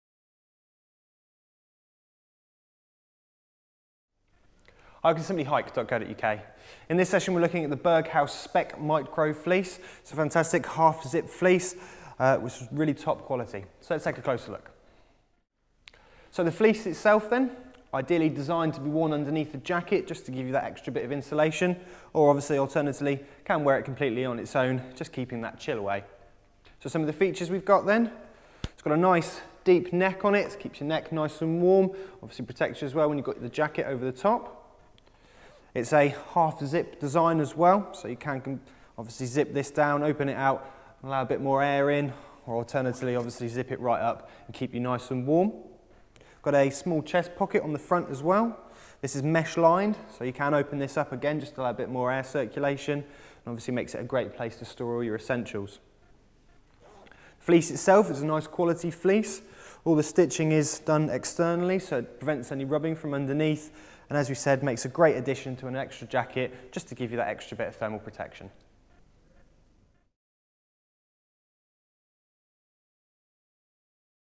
Berghaus Mens Spectrum Micro HZ Fleece
The Spectrum Micro fleece from Berghaus is a half zip, lightweight fleece perfect for trekking, hiking or any other fast activity.